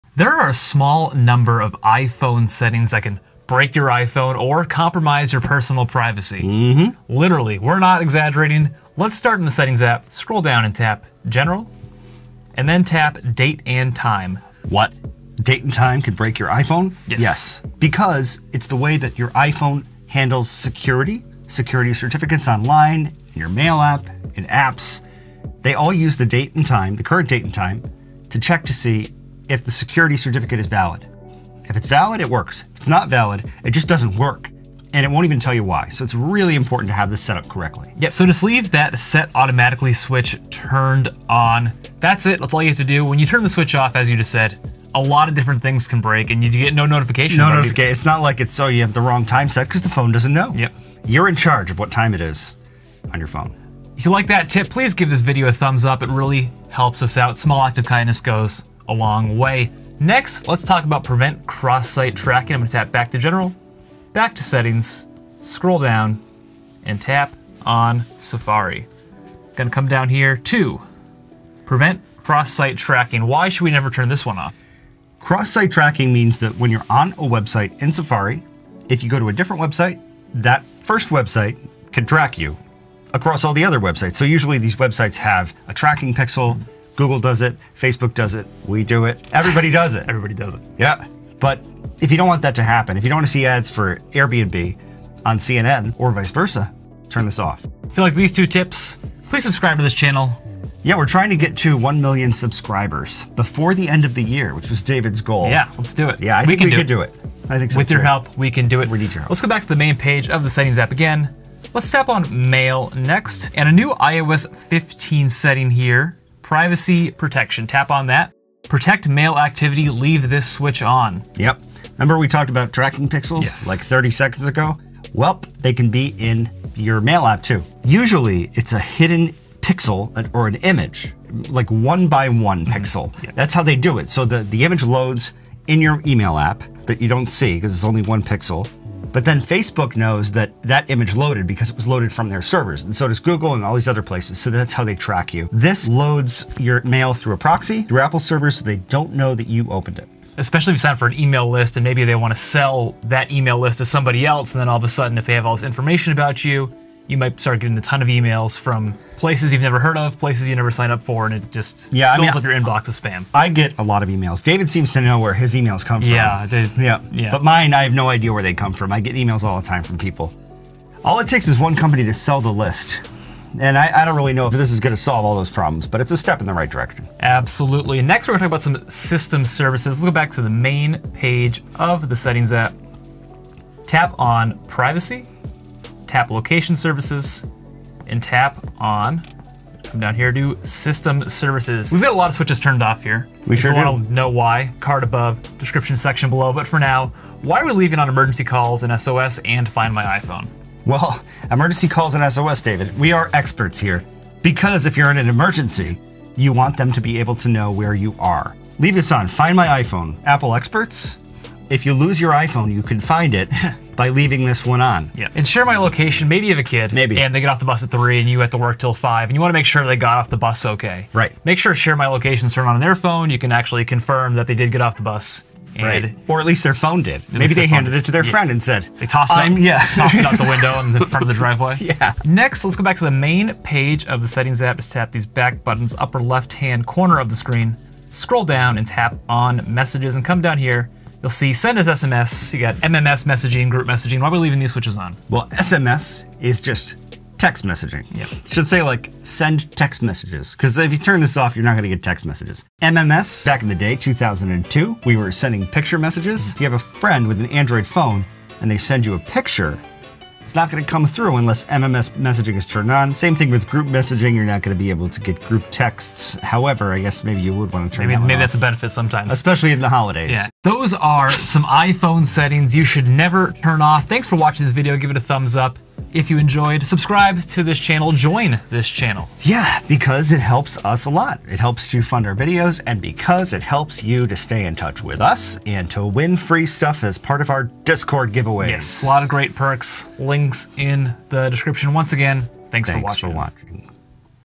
NEVER Turn Off These 5 iPhone Settings
David & David tell you about five iPhone settings you should NEVER turn off. Doing so could literally break your iPhone!

1. Introduction [0:00]
2. Date & Time [0:08]
3. Prevent Cross-Site Tracking [0:59]
4. Protect Mail Activity [1:53]
5. System Services (Mostly) [3:02]
6. Leave SMS & MMS Alone [4:06]